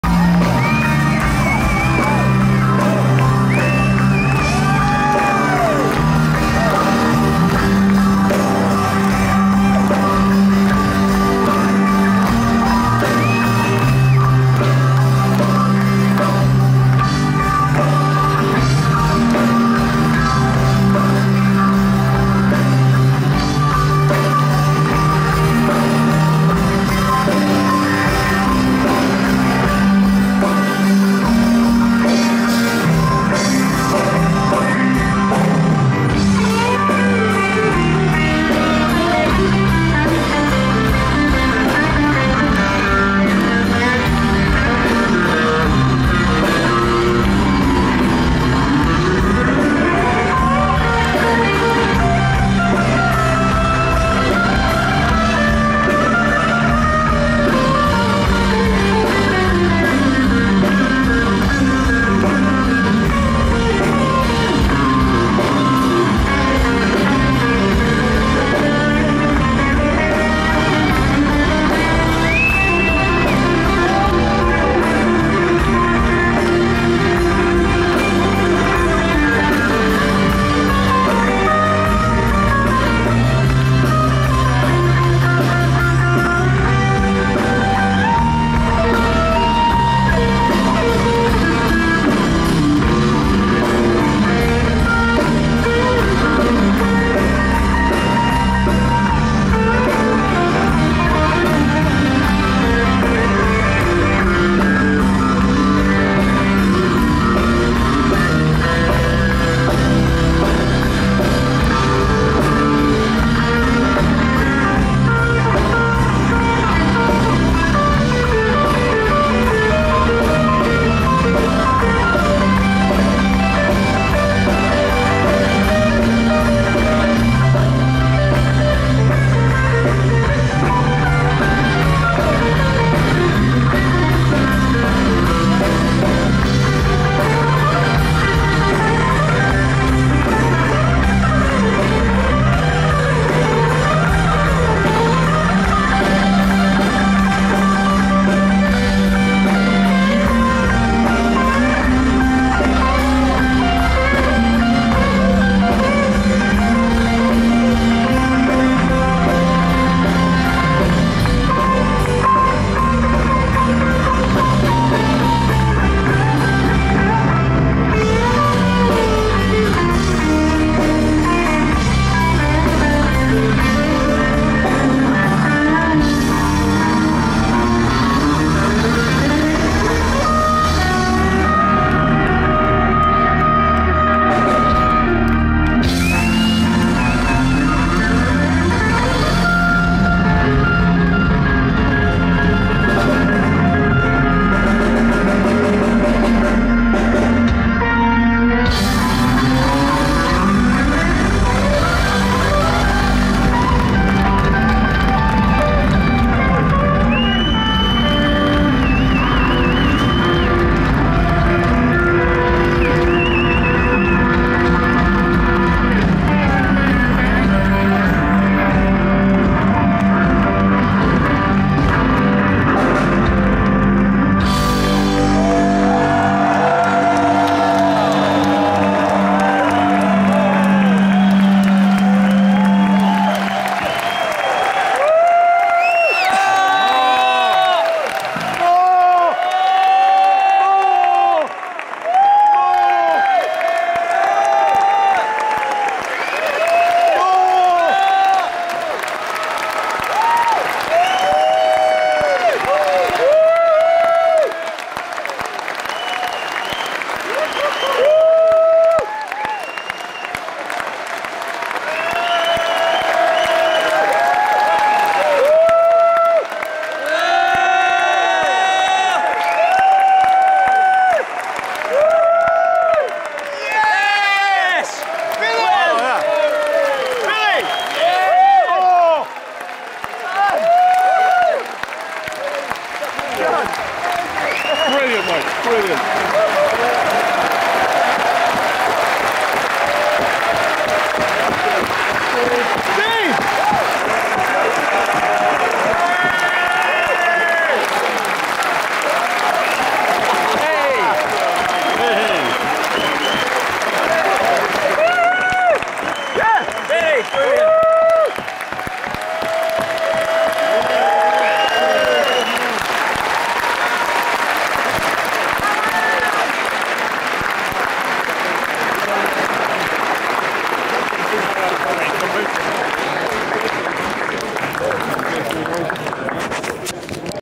YES Live in BRIGHTON 7th May 2016 Close up Encore by rob yalden
YES playing live at The Brighton Centre Sat 7th May 2016.
Encore...See Steve Howe on guitar, Alan White drums, Geoff Downes keyboards, Billy Sherwood bass and Jon Davison vocals.
Five minutes of close up YES genius.
See "yesworld.com" for future concert dates.